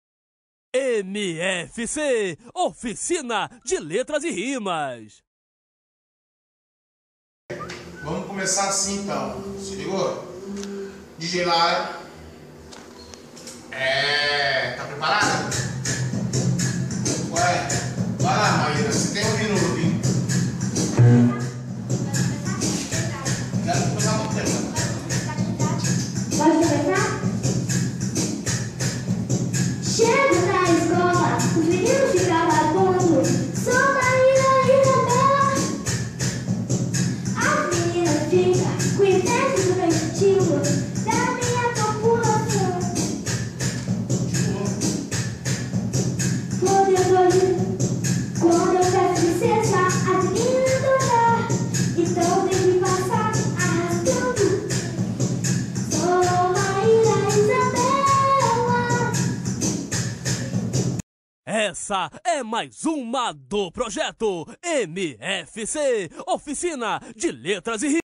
{MFC} OFICINA DE LETRAS E RIMAS NOÇÃO DE COMO CRIA NOTAS E MELODIAS COM LETRAS PARTE 3 MC MAYRA D
As oficinas são feitas em domingos alternados no Bairro Aparecida em Cariacica-ES, na casa do MC Sapão Funk Capixaba e MC Rosana Morena